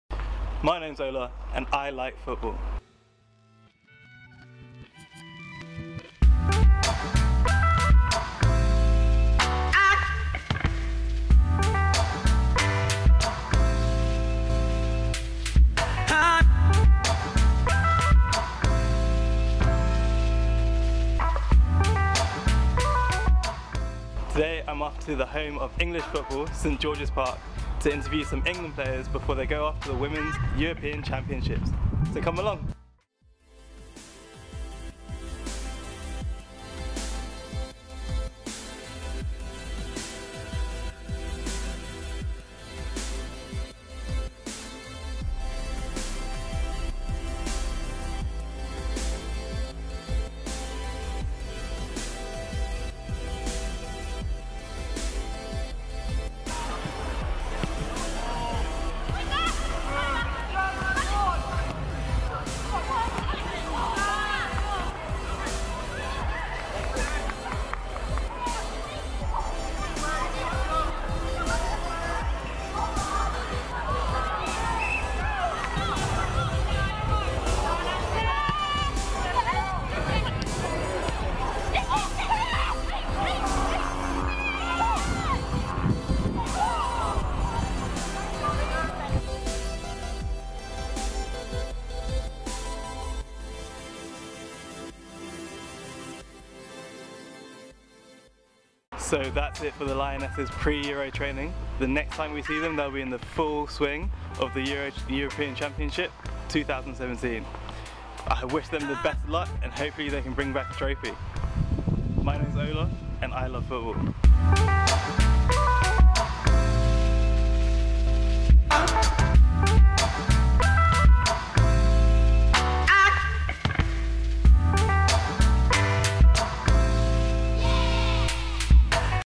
FOOTBOLA Ep 10: Lionesses!!
This week I'm off to the home of English football, St George's Park.

Follow me as I meet the latest and greatest English football team, the Women's National Team, as they prepare for the European Championships.